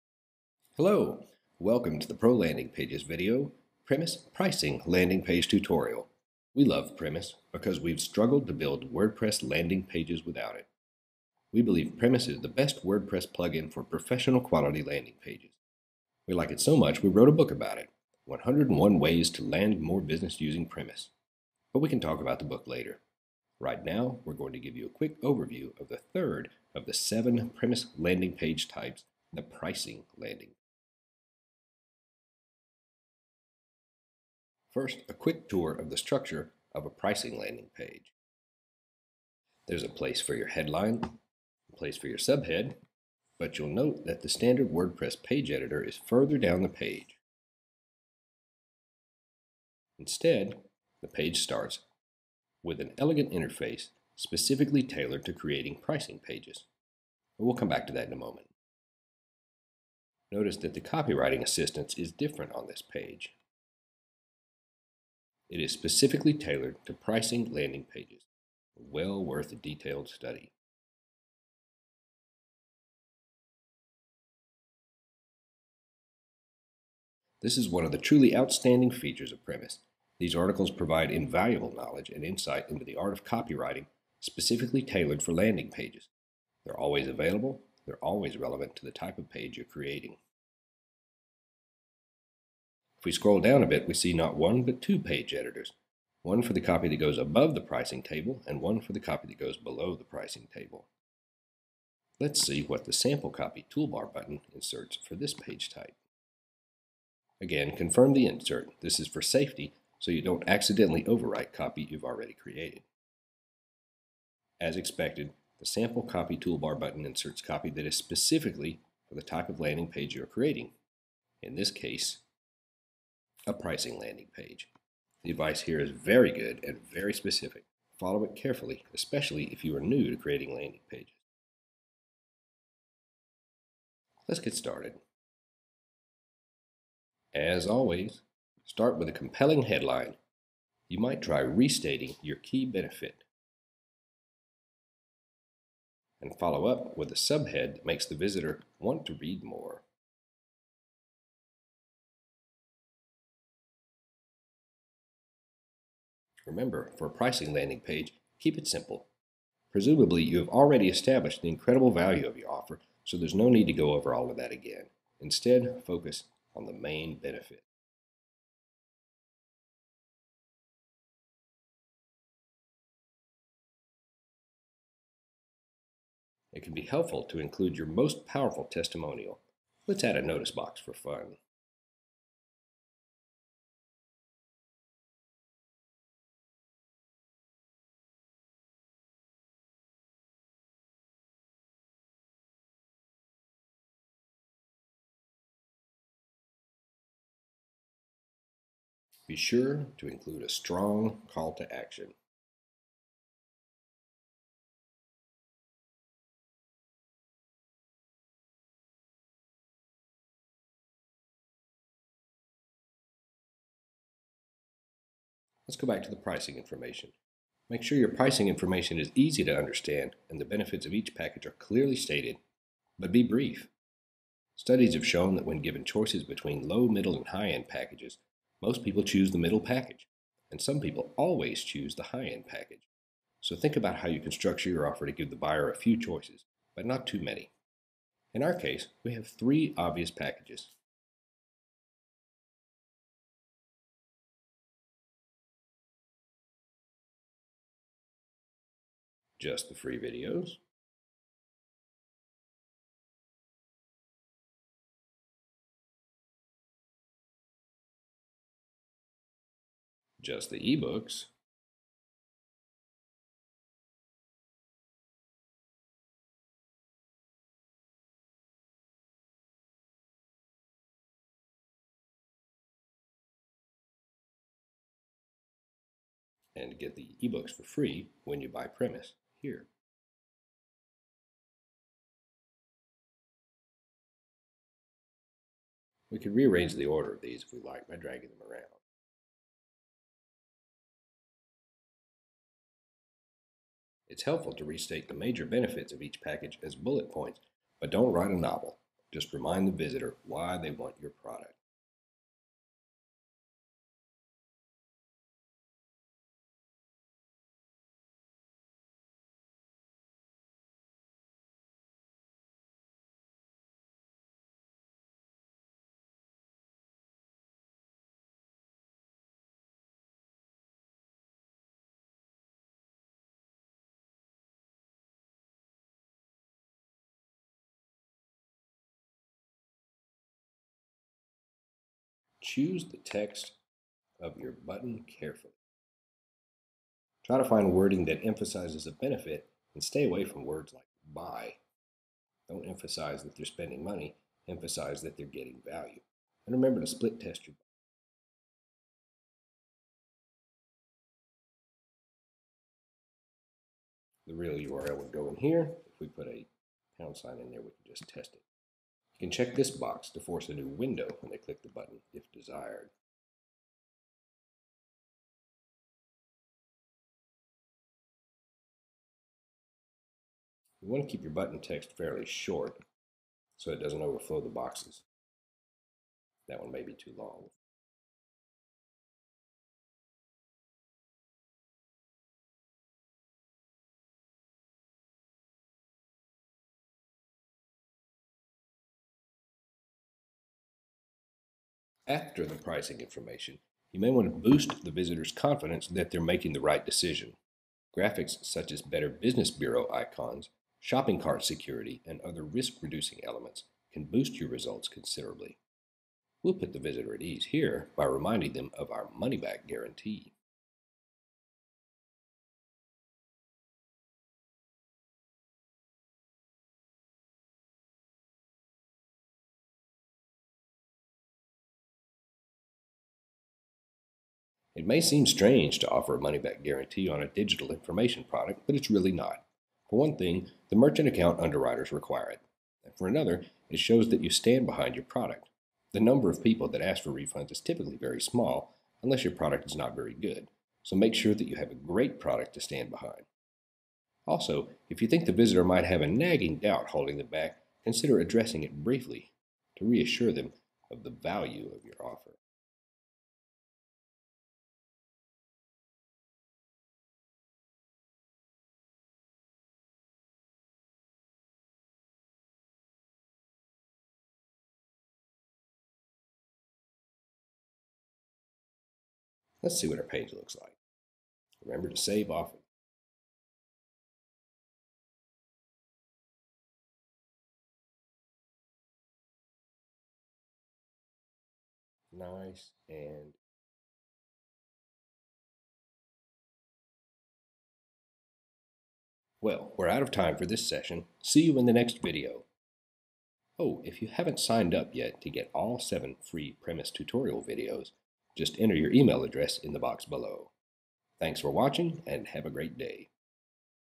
Premise Video 3 of 7 - Pricing Landing Page
The third of 7 video tutorials on Premise, the best WordPress plugin for professional-quality landing pages.